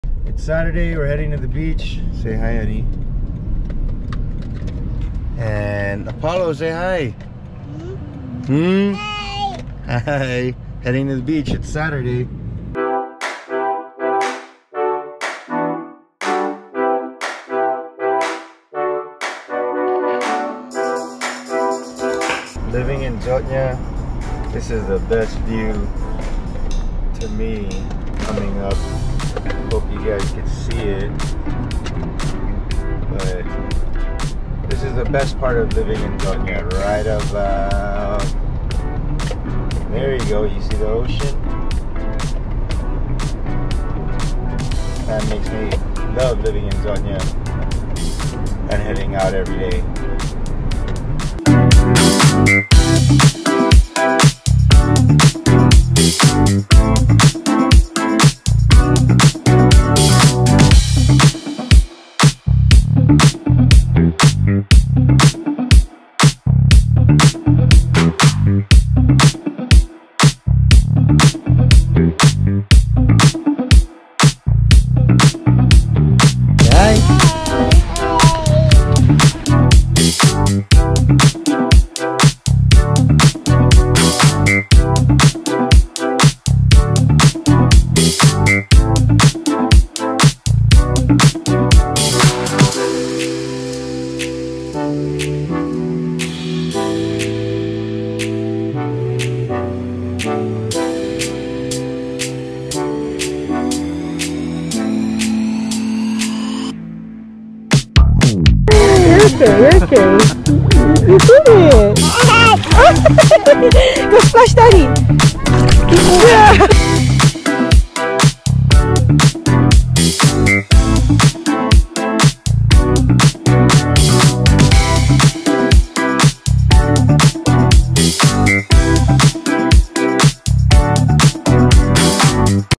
Family Beach Time | Ypao Beach | Guam
Family beach time at Ypao beach.

Video shot on GoPro 11